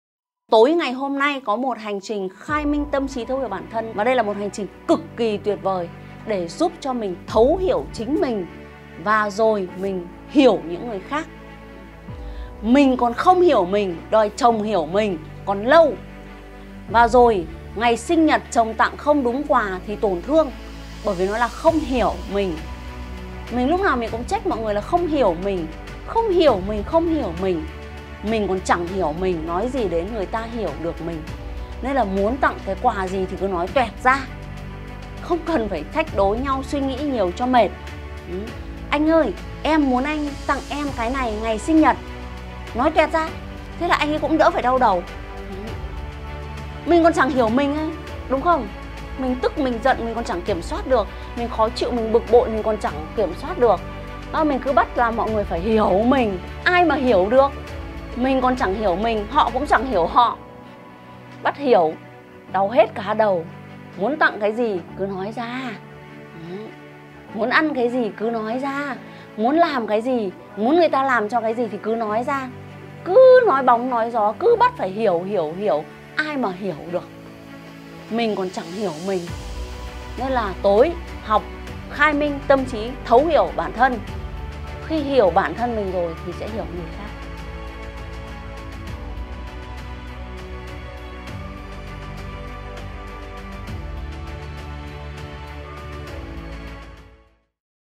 [Tham gia ngay] Chương trình KHAI MINH TÂM TRÍ tổ chức miễn phí tối nay!!
Tối nay chương trình KHAI MINH TÂM TRÍ Buổi 1 sẽ diễn ra vào 20h00.
Cả nhà mình cùng tham gia Group "CỘNG ĐỒNG CHA MẸ THÔNG THÁI" để nhận link vào phòng ZOOM nhé!